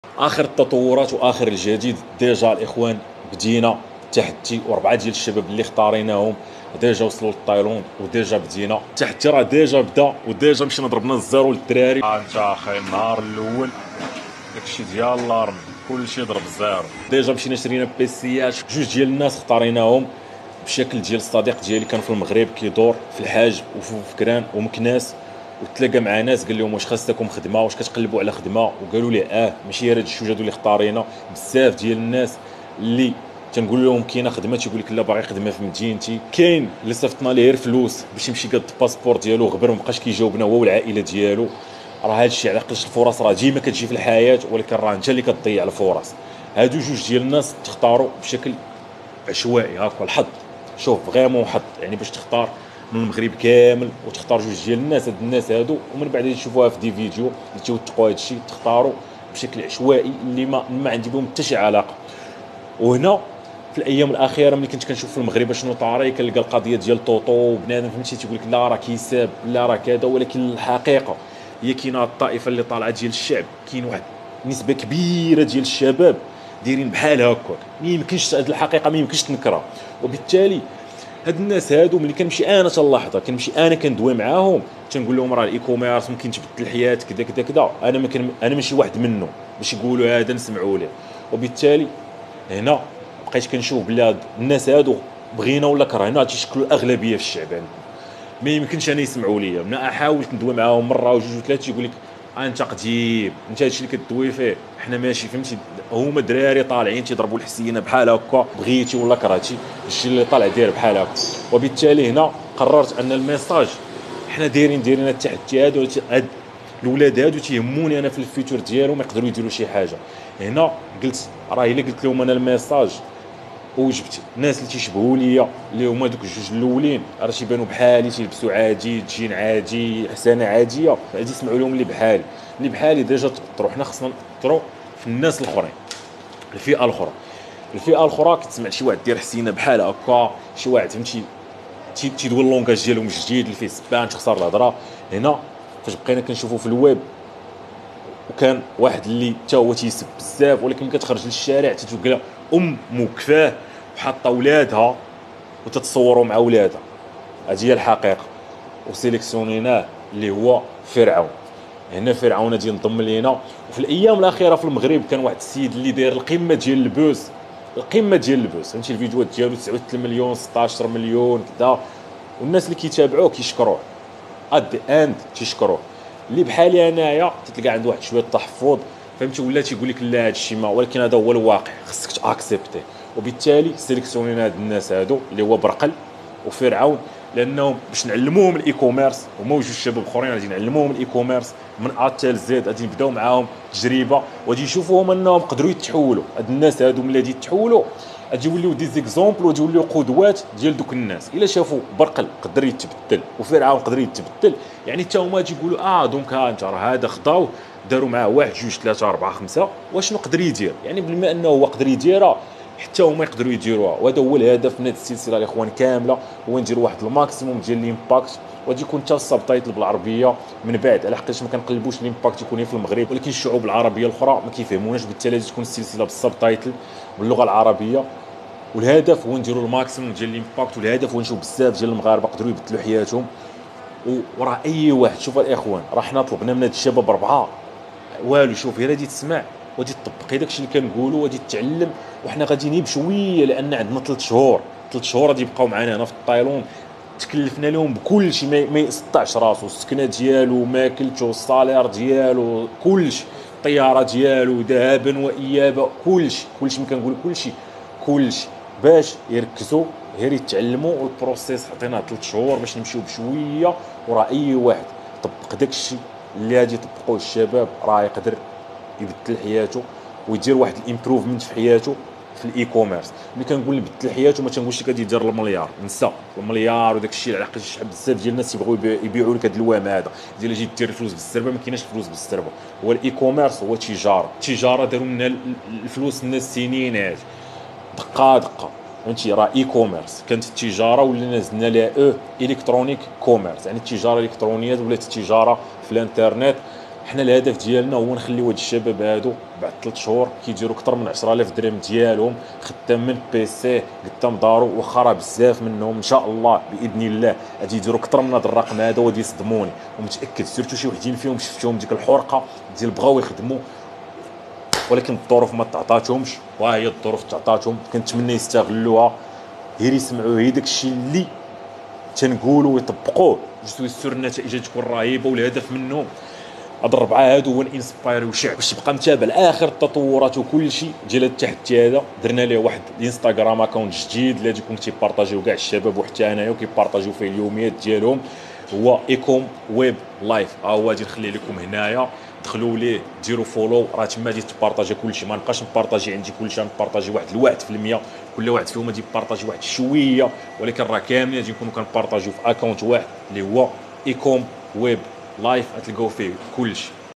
Simolife,fir3awn,br9l hotgam,Toto l سلسلة مليونير من الزيرو 🤯 ,سيمولايف, برقل هوتغام,فرعون,طوطو
Simolife, fir3awn, br9l hotgam, Toto l كل شخص يستطيع ان يغير حياته, سيمولايف, برقل هوتغام,فرعون,طوطو

simolifedotcom
تم اختيار اربع اشخاص اول واحد كان يشتغل في مجال البناء بناي من مكناس و الثاني كان يدرس من الحاجب و اثنين من اكبر المبلبلين في الويب المغربي و الذي يقال عليهم ان مفقود فيهم الامل ويستحيل ان يتم تغيير هؤلاء ولكن سيمو لايف سيثبت باذن الله ان المستحيل ممكن، خلال ثلاث اشهر سيقضون معه في تايلاند سيجعل منهم شخصيات ناجحة و قدوات يضرب بهم المثل ليغير مستقبلهم بشكل كامل بهدف ان يثبث ان كل شخص يستطيع ان يغير حياته، دعواتكم معنا